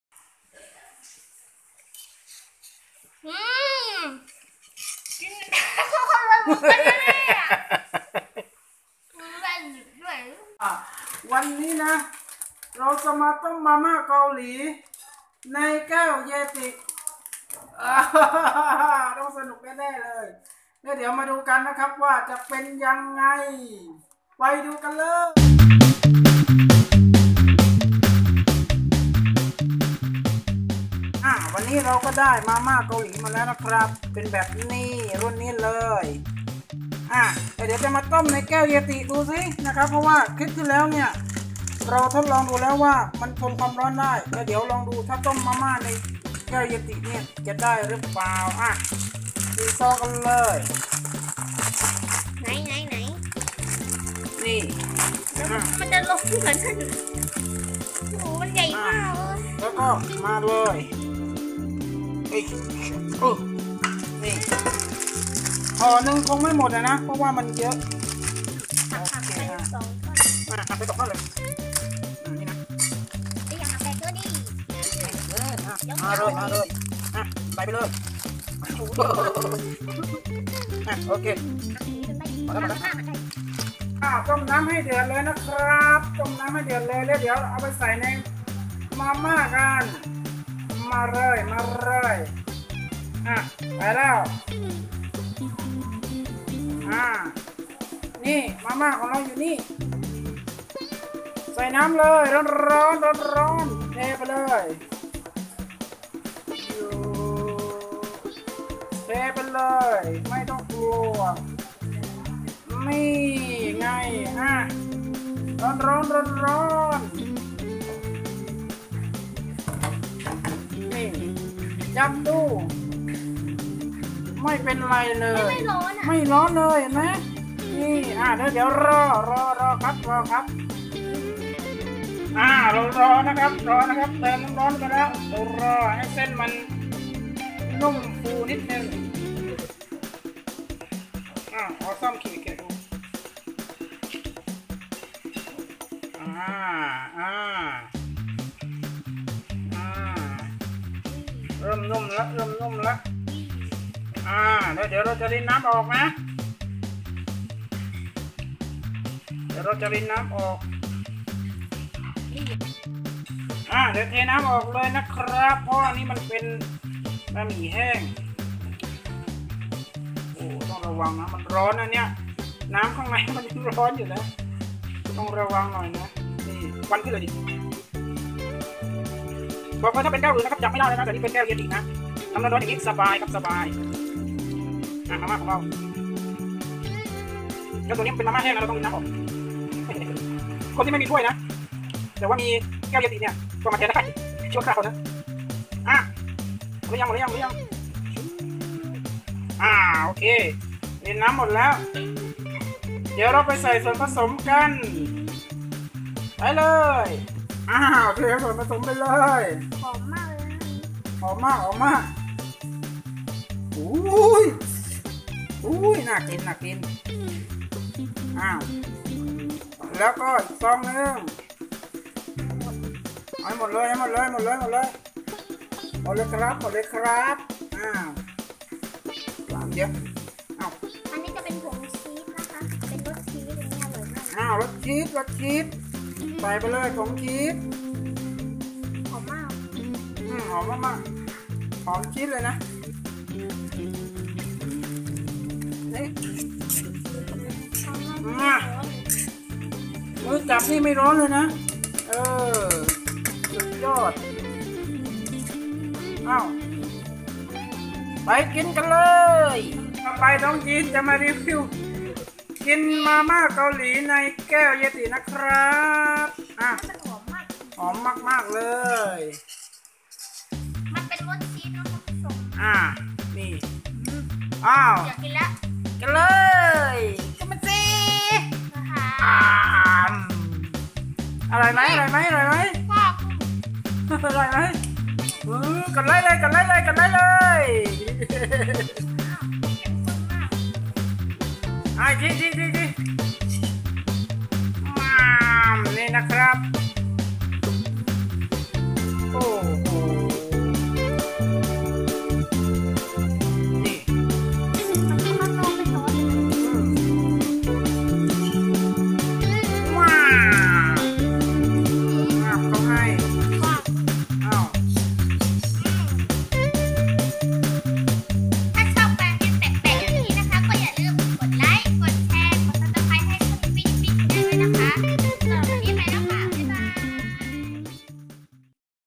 รีวิวแก้ว YETI ต้มมาม่าเกาหลี YETI Rambler Review
จะเป็นยังไง ถ้าเอาแก้ว YETI มาต้มมาม่าเกาหลี ไปดูกันเลย